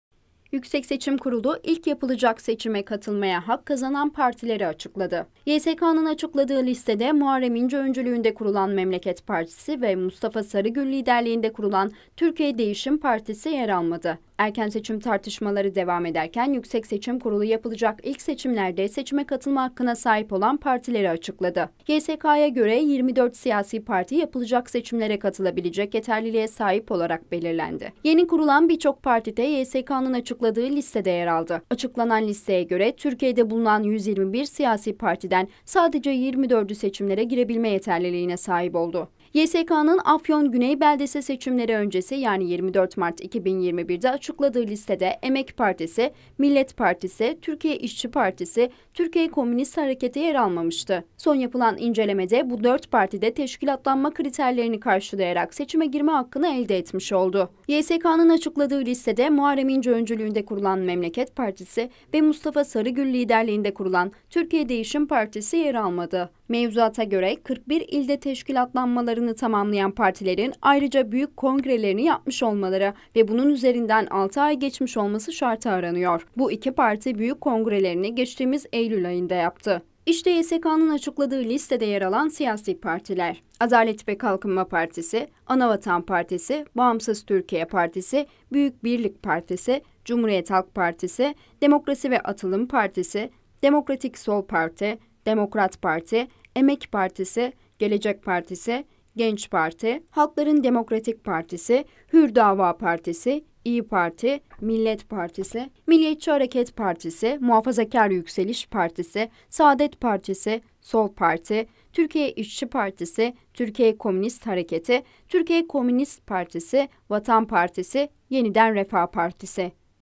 YKS'dan Muharremİnce ve MustafaSarıgül'e şok karar
Yüksek Seçim Kurulu (YSK) ilk yapılacak seçime katılmaya hak kazanan partileri açıkladı. YSK'nın açıkladığı listede Muharrem İnce öncülüğünde kurulan Memleket Partisi ve Mustafa Sarıgül liderliğinde kurulan Türkiye Değişim Partisi yer almadı...